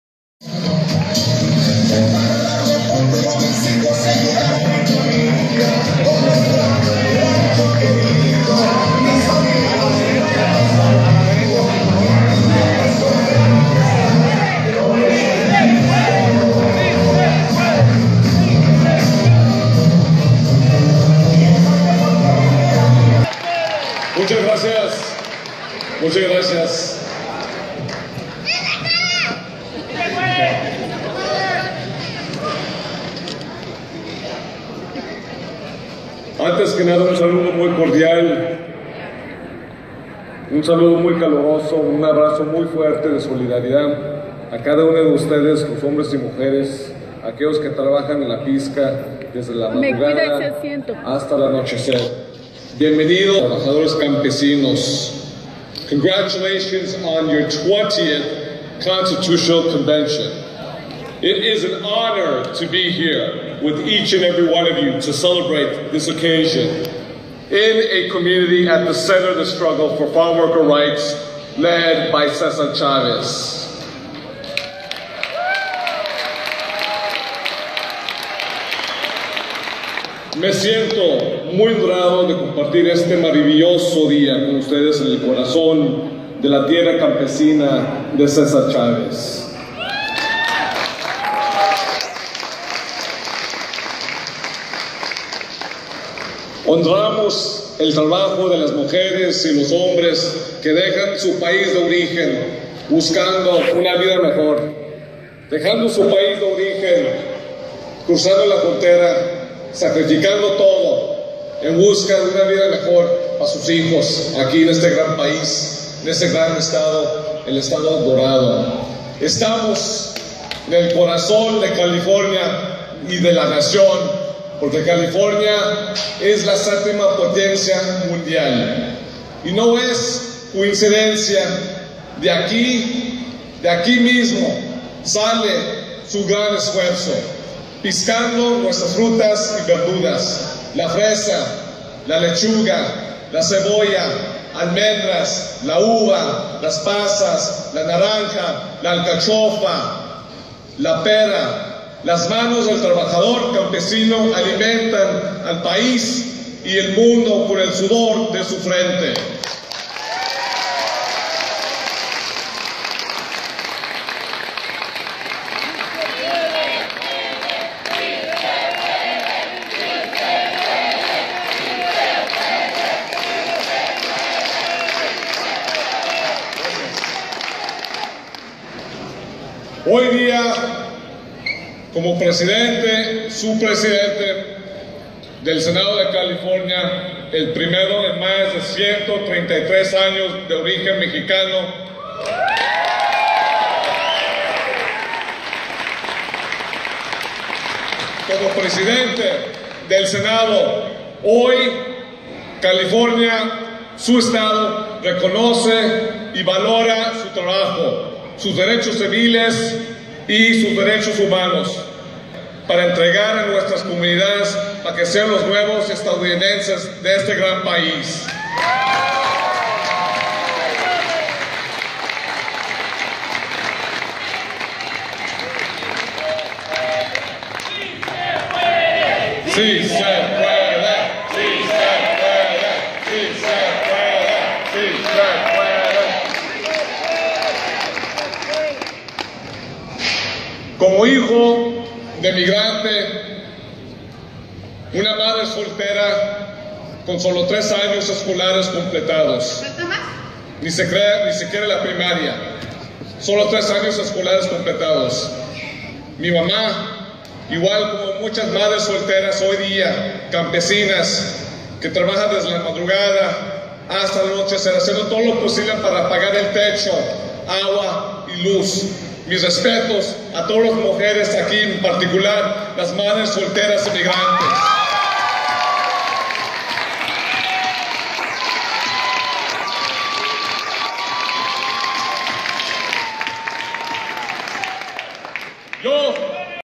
California State Senator Kevin de León at the UFW 20th Constitutional Convention - ufw2016
El senador Kevin de León en la 20a Convencion Constitucional de la UFW